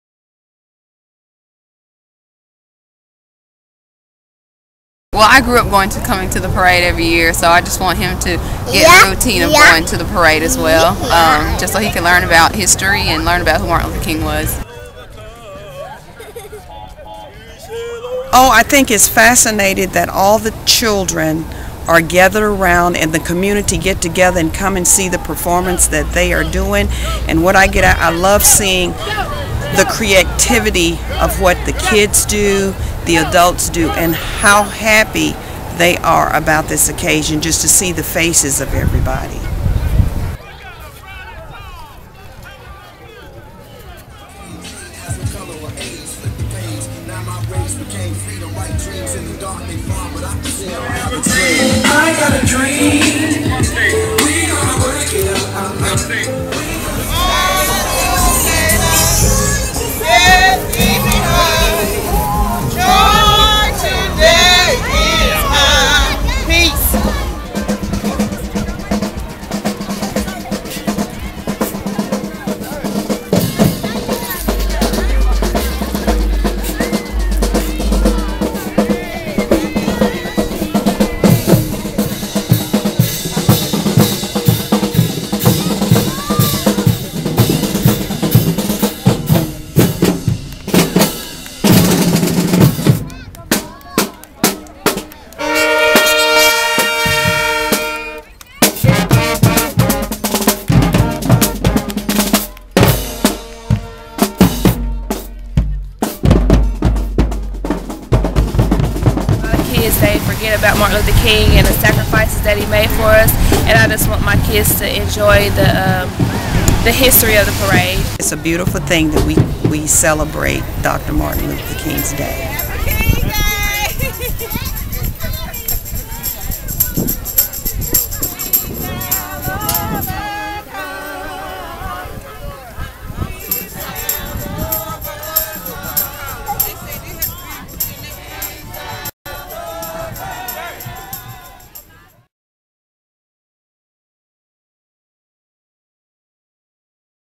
Sights and sounds from the 2014 MLK Parade
Churches and children, sororities and Shriners, puppets and politicians, mayors and musicians came together to commemorative the life of Dr. Martin Luther King Jr. with a tribute parade in Knoxville, Tenn on Jan. 20, 2014. This year's theme was "Moving forward, claiming our greatness." (Video by Kristi Nelson Bumpus/News Sentinel)